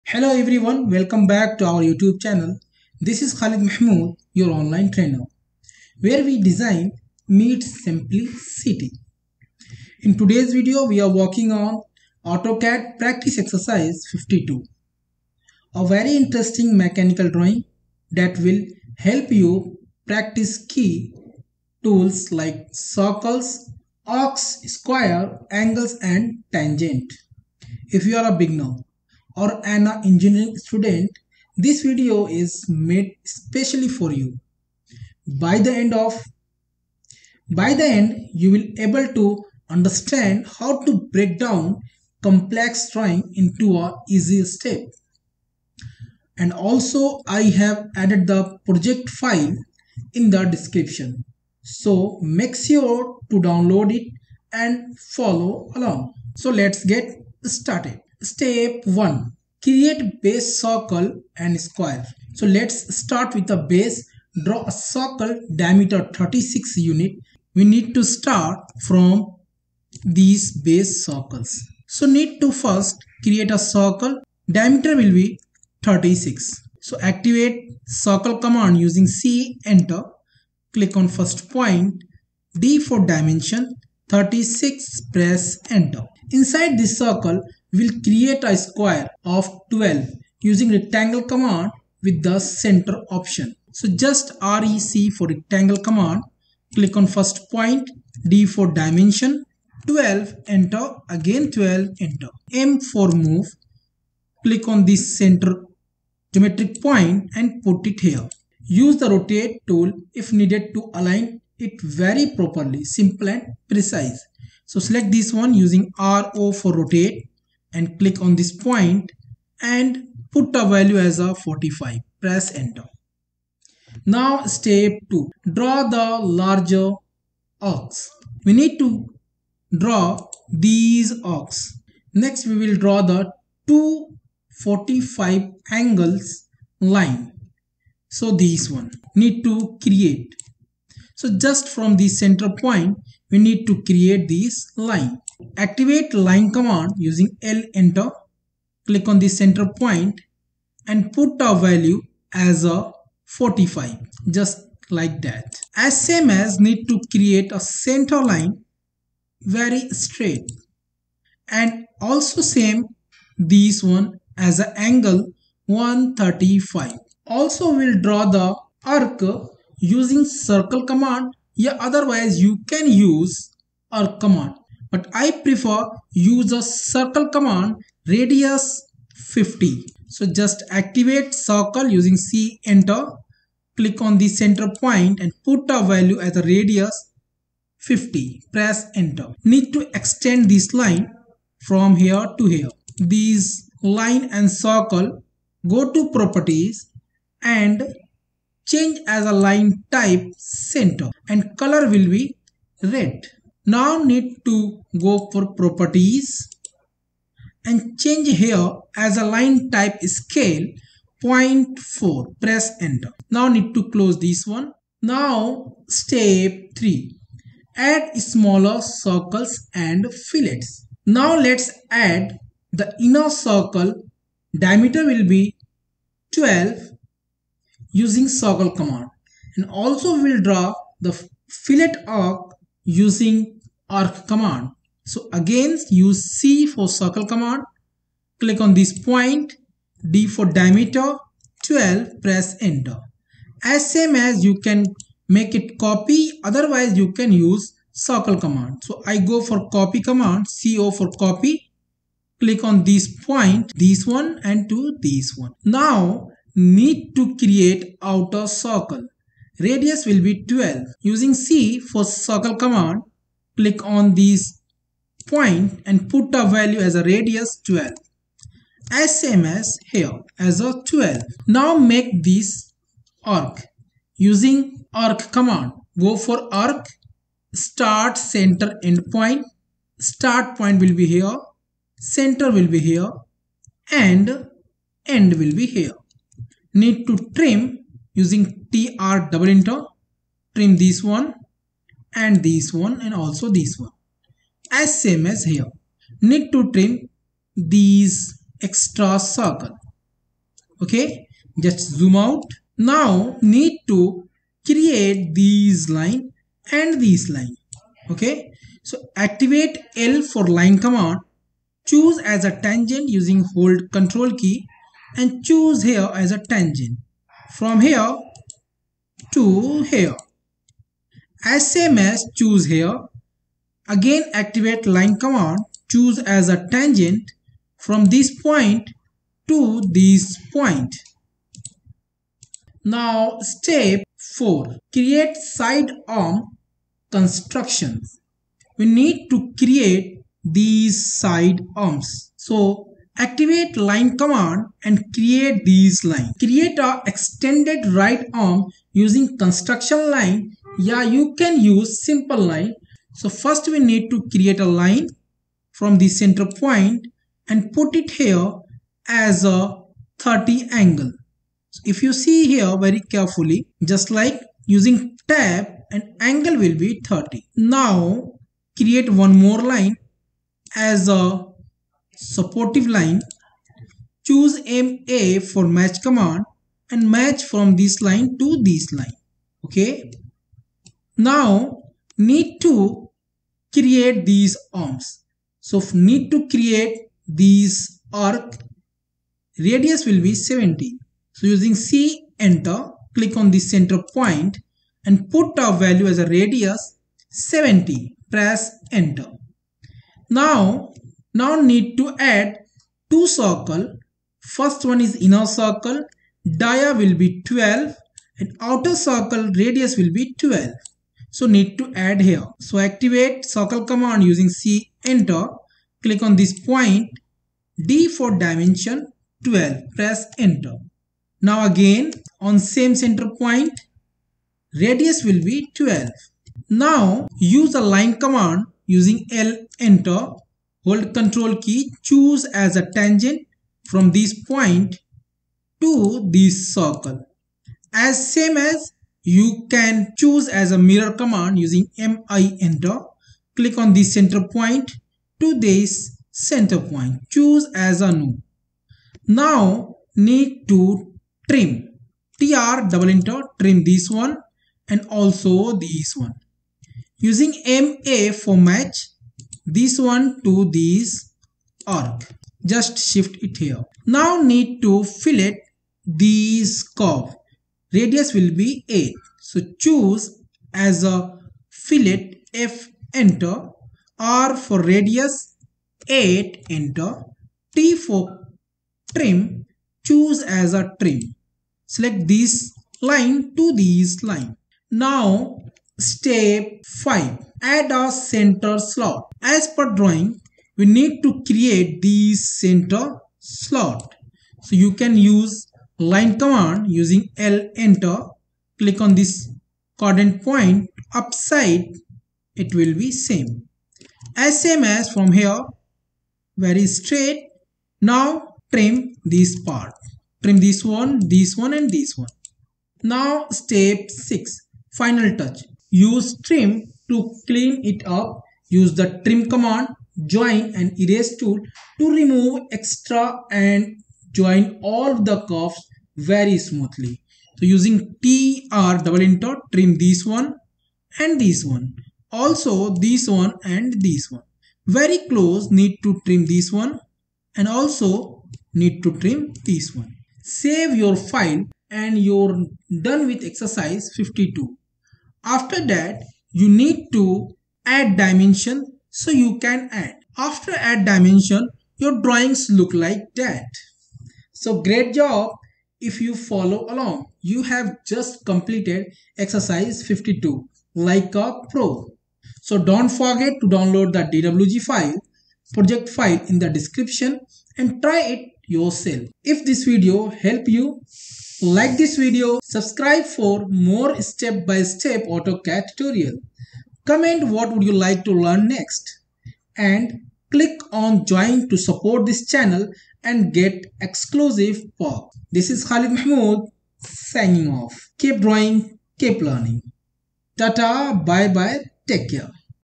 AutoCAD 2D Practice Exercise 52 | Step-by-Step Drawing Tutorial (Beginner Friendly)
🎓 Learn AutoCAD the Easy Way!
In this step-by-step tutorial, we will create AutoCAD Practice Exercise 52 from scratch. This 2D mechanical drawing is perfect for engineering students, beginners, and anyone looking to improve their AutoCAD drafting skills.
We’ll walk you through circles, arcs, fillets, angles, and more — using basic AutoCAD commands like CIRCLE, ARC, FILLET, TRIM, RECTANG, and ROTATE.
🧑‍💻 This video is in simple English and is easy to follow, even if you're just starting out with AutoCAD.

▼ LET’S CONNECT For Latest Updates ▼